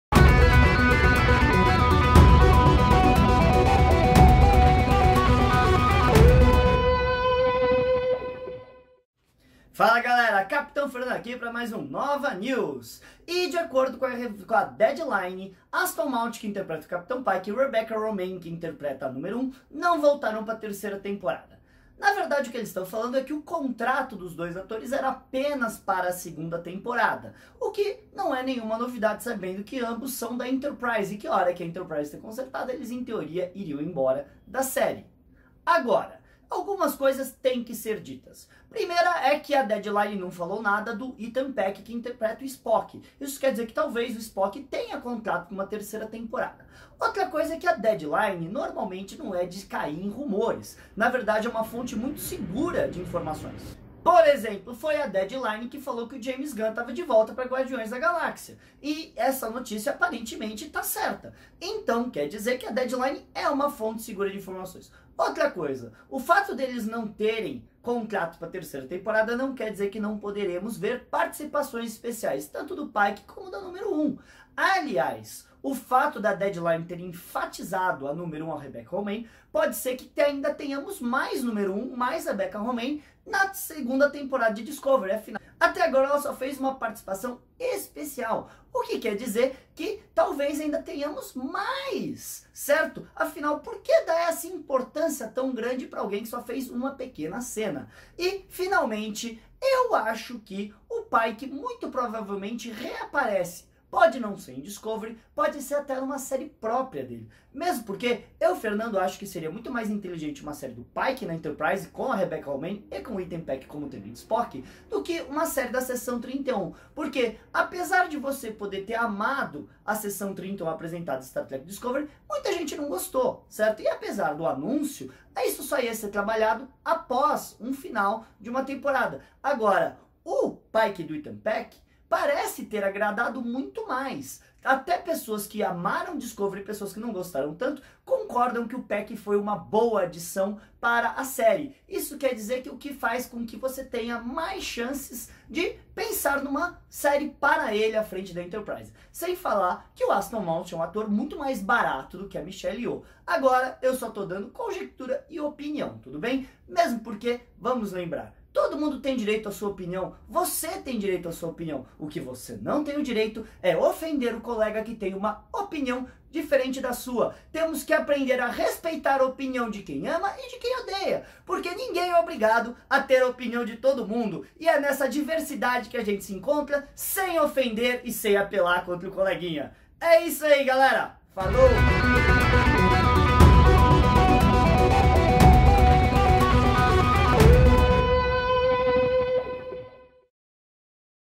Capitão Pike Fora de Star Trek Discovery - NovaNews
Capitão Fernando comenta a notícia de que o Capitão Pike e a Numero 1 não voltam para a terceira temporada de Star Trek Discovery.